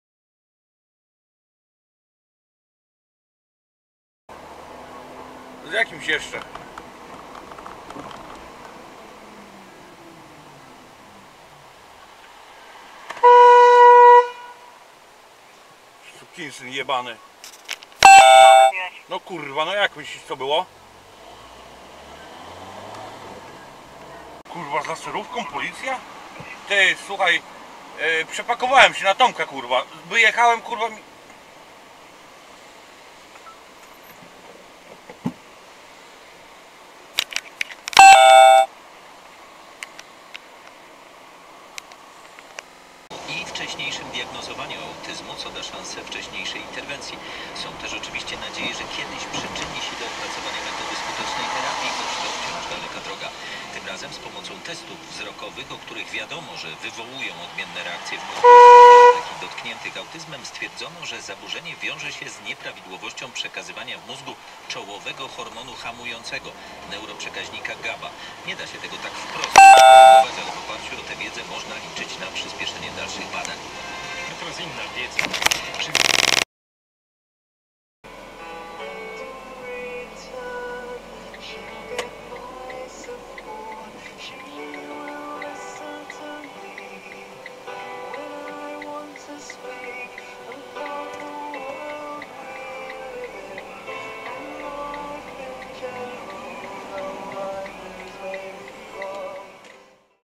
Niebezpiecznie na pasach HD
Po potrąceniu na pasach będziesz miał długi czas na przemyślenia , jednosekundowego zachowania na drodze !!!